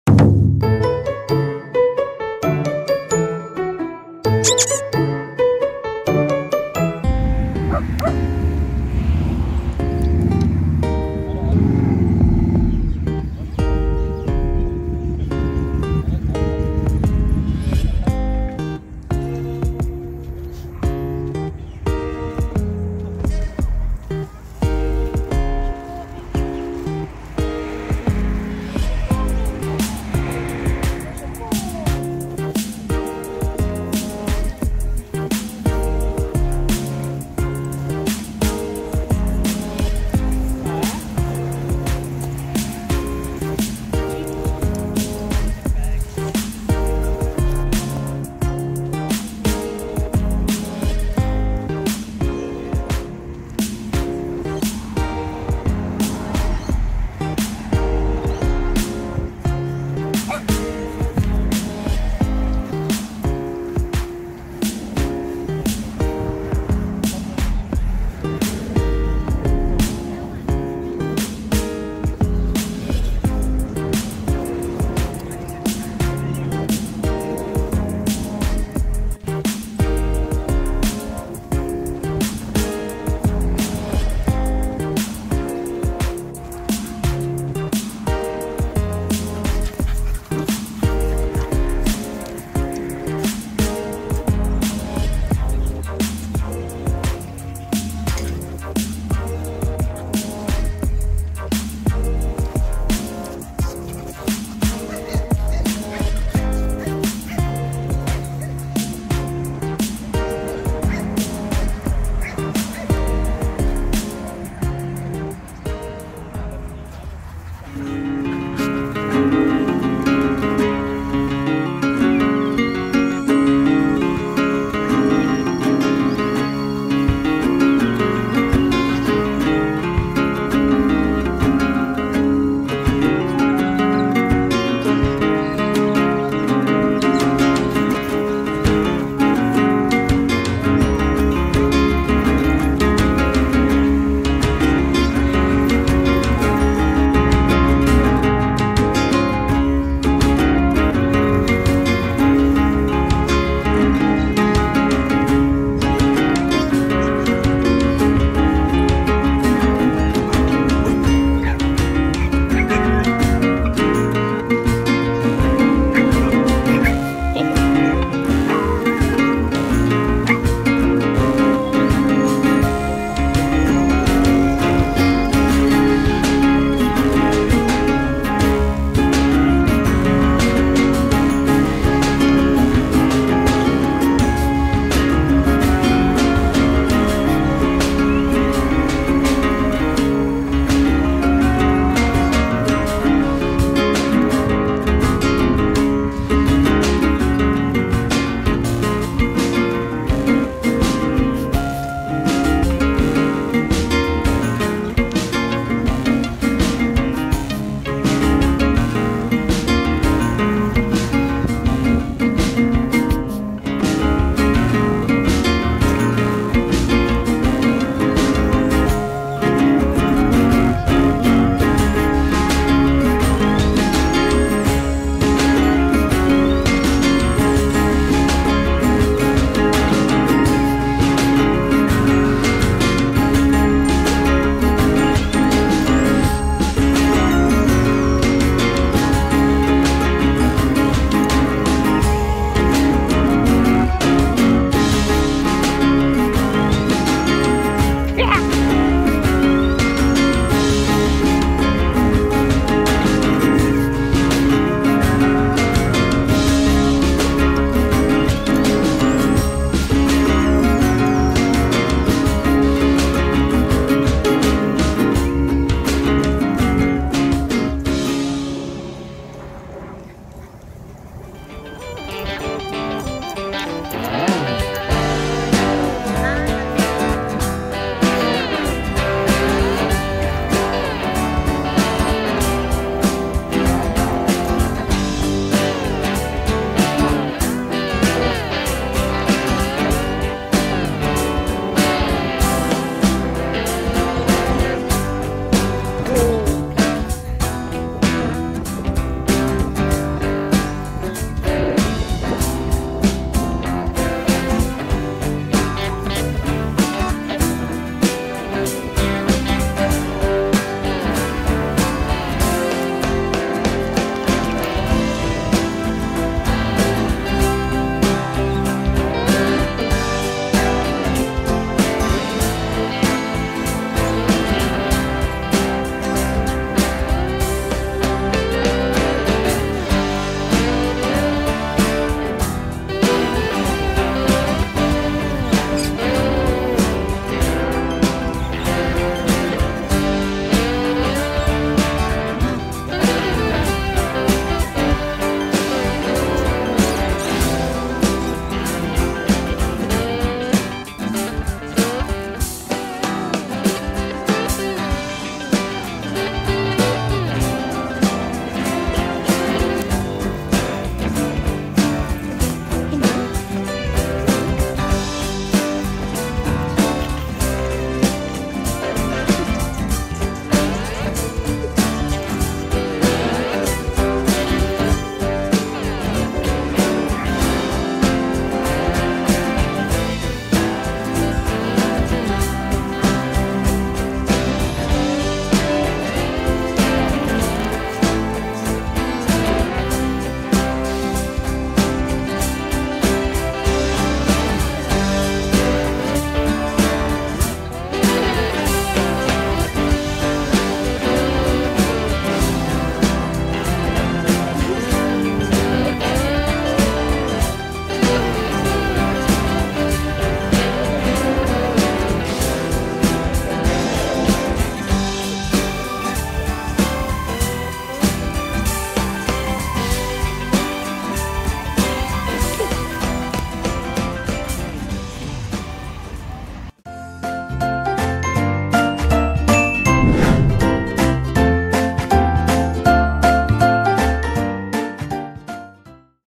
TV for DOGS | DOGTER COFFEE Jack Russell Terrier
Hello, Aunties and Uncles. my name is Coffee ☕ 🐶
I am a Jack Russell Terrier and I am very cute and adorable girl 🥰